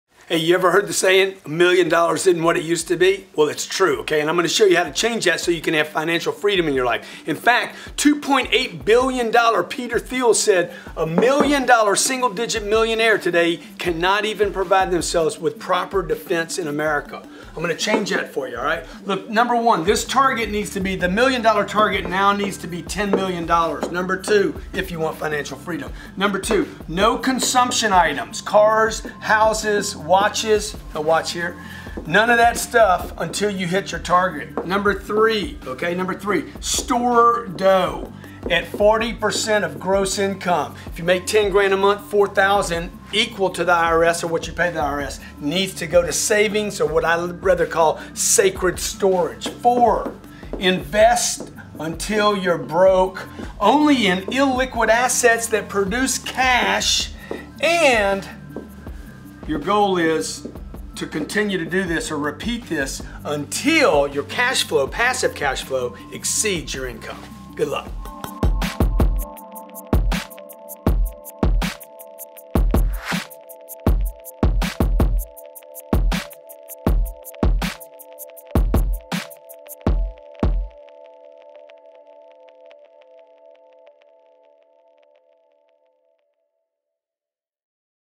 How to Reach Financial Freedom - Grant Cardone
hese are the exact 5 steps that I used to not only become a millionaire, but to accumulate hundreds of millions of dollars in my lifetime.

Take notes, and share your success once you begin to implement these strategies into your proccess.

Post in comments.